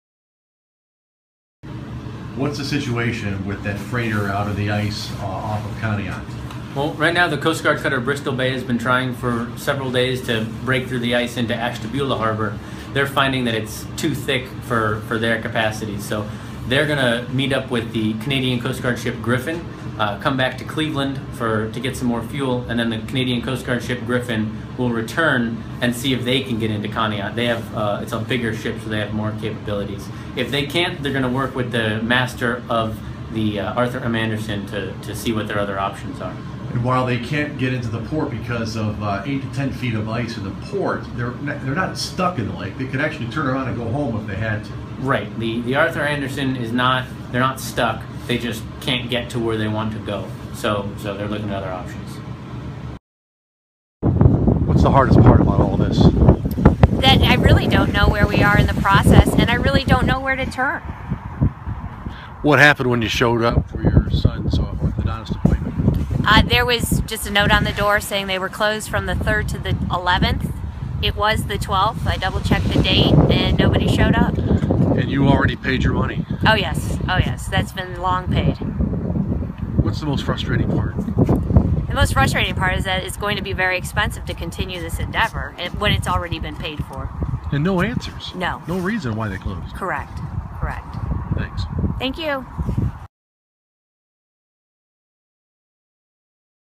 Reporter Paul Kiska speaks with Lt. Davey Connors from USCG
A Coast Guard ice cutter is attempting to free a freighter stuck in the ice on Lake Erie.